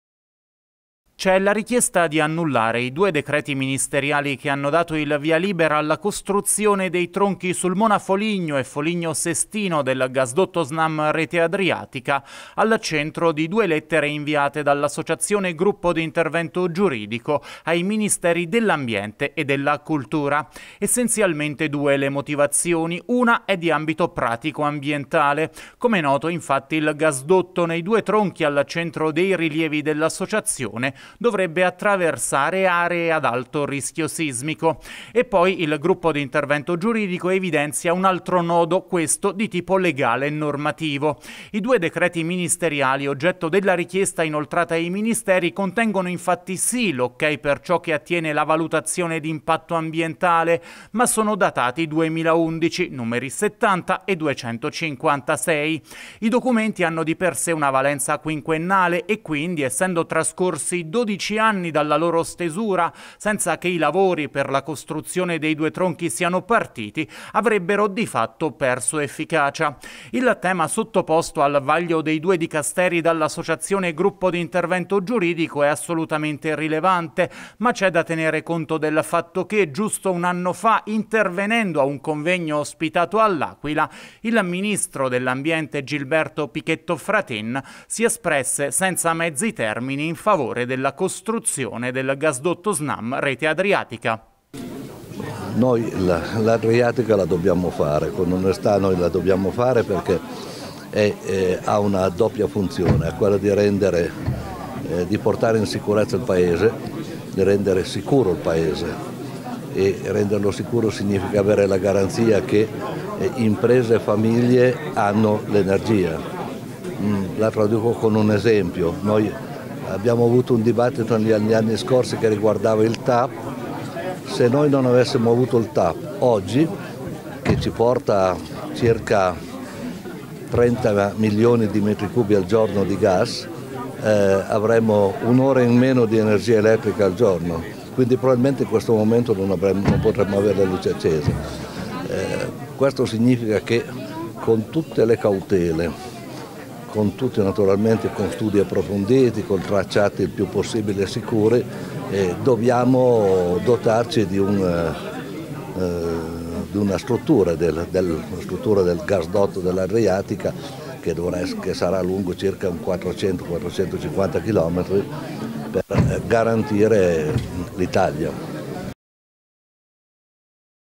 Gasdotto SNAM, Gruppo d'Intervento Giuridico scrive a due Ministeri
L'associazione ecologista gruppo di intervento giuridico scrive ai ministeri dell'ambiente e della cultura.

Al centro il tema del gasdotto SNAM.

La richiesta: annullare i decreti con l'ok per i tronchi Sulmona-Foligno e Foligno-Sestino.

Di Angelo Liberatore